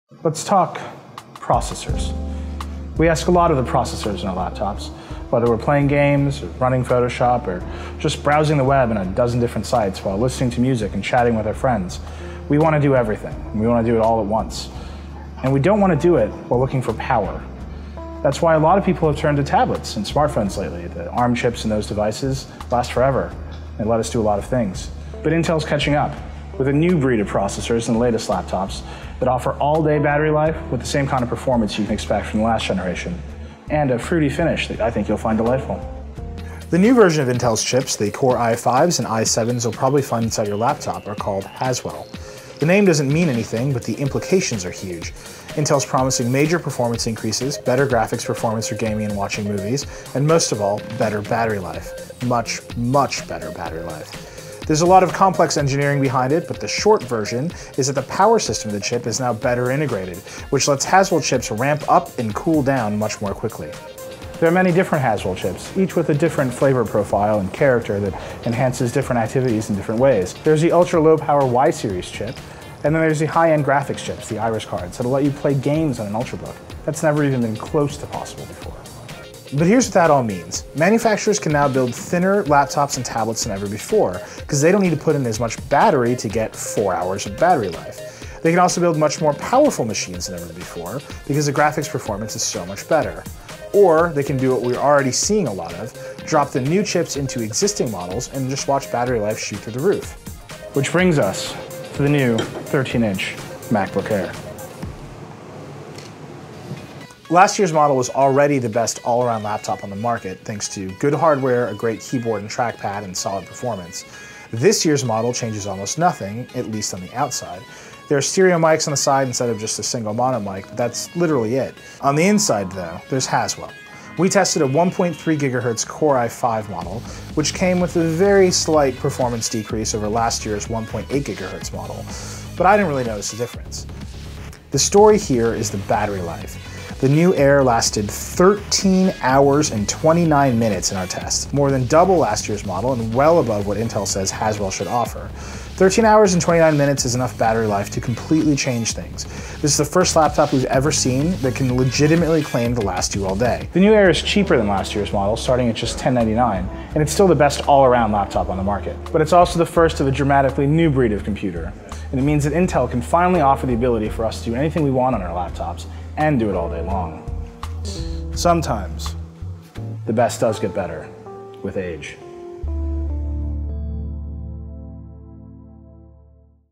Top Shelf: Haswell and the 2013 MacBook Air
Nilay Patel describes the benefits of Intel's newest CPU in the 2013 MacBook Air. More from The Verge: